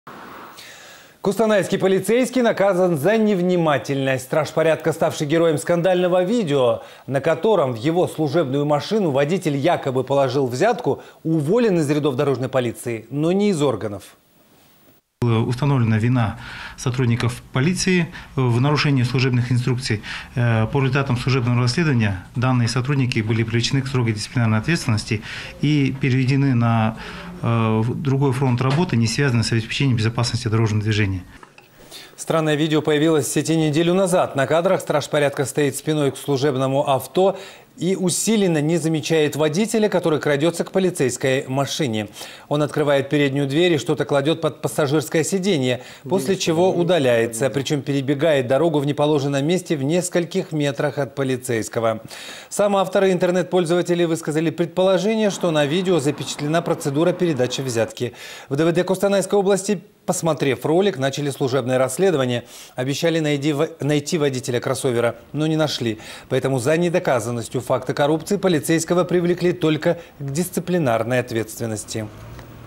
Костанайский полицейский наказан за невнимательность
Костанайский полицейский наказан за невнимательность. Страж порядка, ставший героем скандального видео, на котором в его служебную машину водитель якобы  положил взятку, уволен из рядов дорожной полиции, но не из органов.